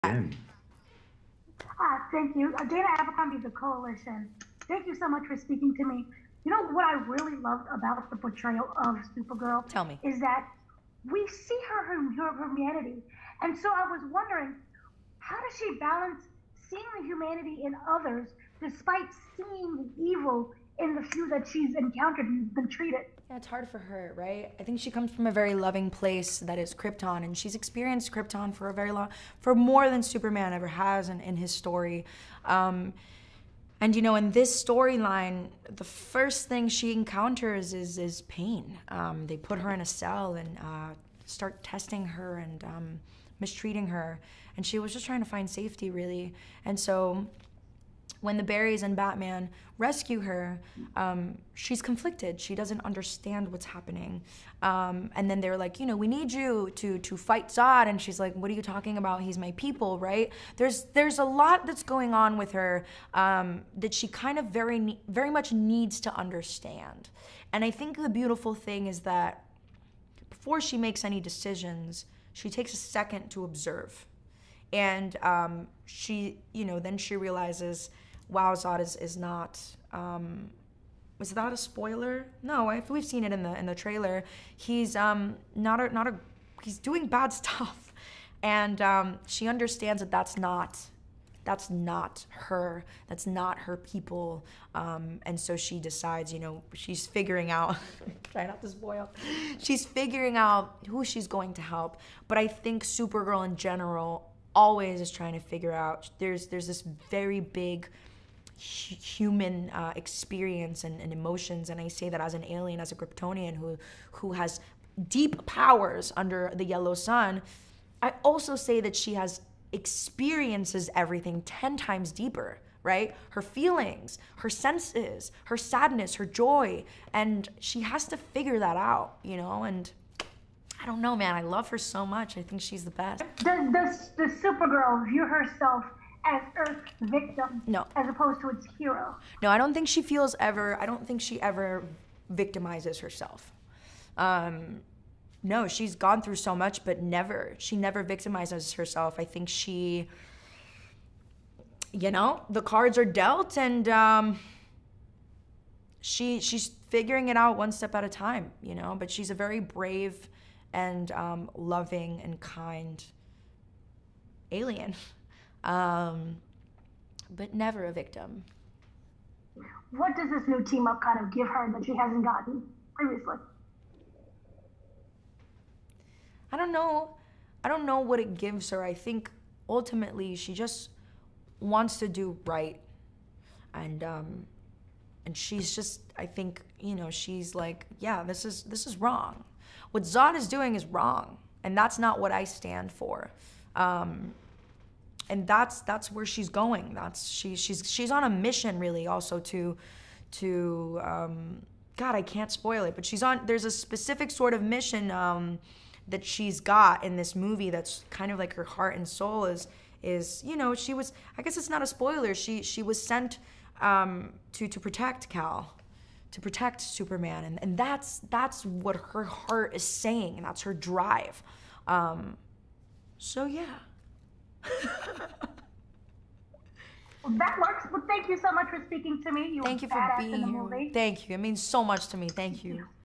The Flash's Sasha Calle On The Humanity of Supergirl / Kara Zor-El & Protecting Superman
Our Entertainment Editor spoke with actress Sasha Calle to learn more about her depiction of Kara Zor-El / Supergirl, finding humanity in the character, and more.

The Koalition provides videos and podcasts relating to the latest news in both gaming and pop culture.